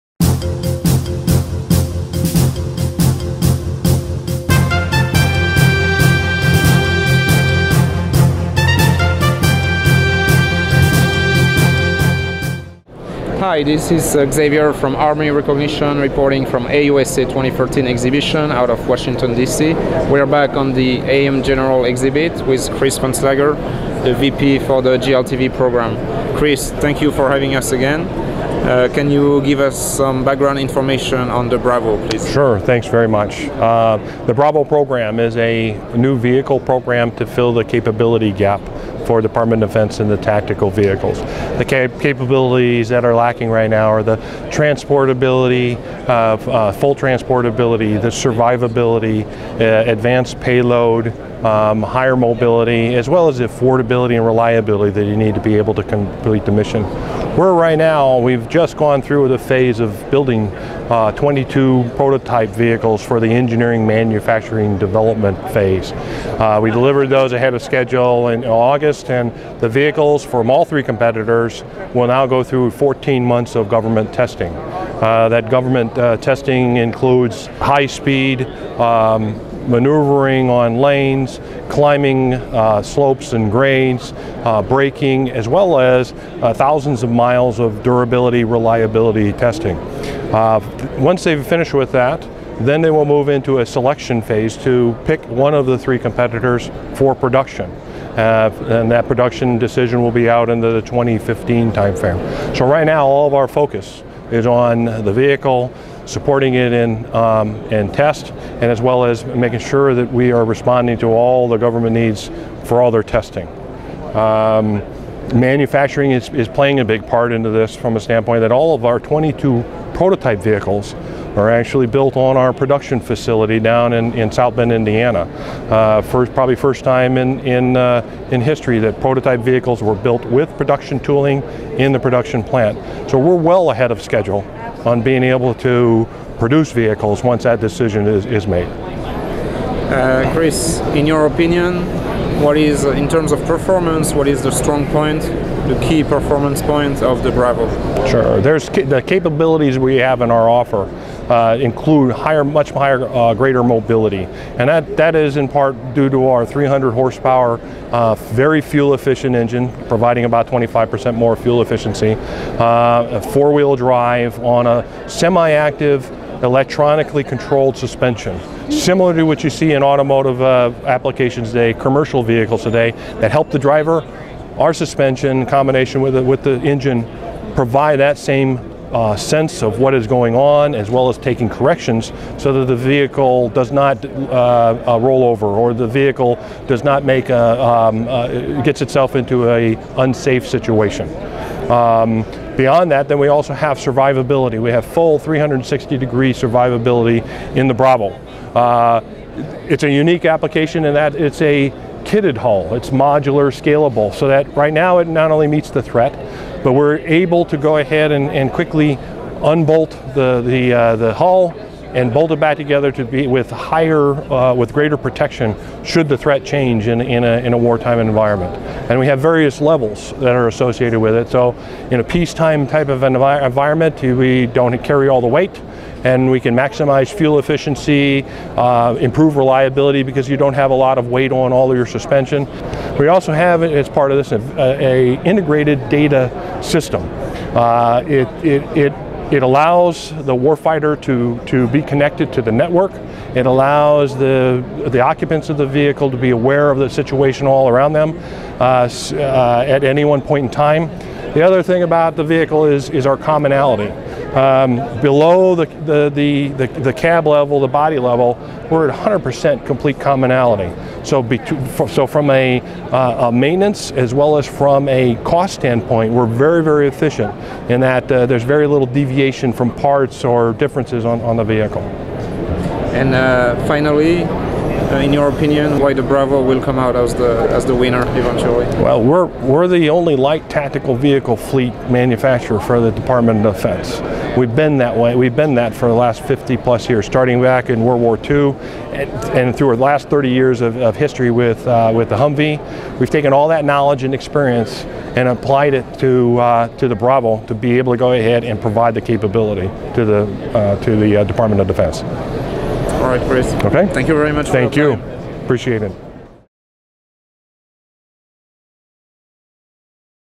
AM General BRV-O interview with Vice President on JLTV at AUSA 2013
The U.S. Defense Company AM General which has manufactured more military
light tactical vehicles than any other in the United States is poised to produce the Joint Light Tactical Vehicle (JLTV), the next-generation Light Tactical Vehicle (LTV) for Soldiers, Marines and other American service members performing their missions around the world.